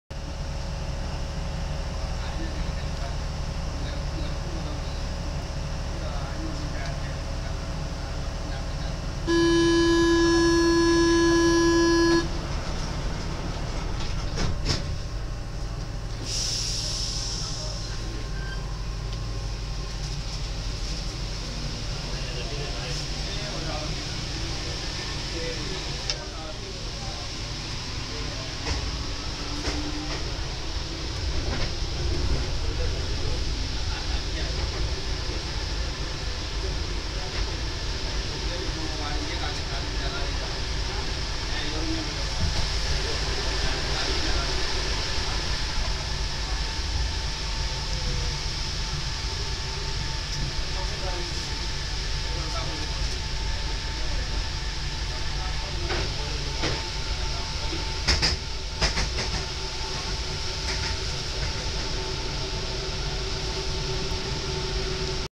Line 1 of Paris metro, off the tunnel
The part of line 1 of Paris metro between the second and third station, as it's seen from the first seats!

Le part de la ligne 1 de métro de Paris entre la deuxiéme et la troisiéme station, comme il se voit des prémiers sièges!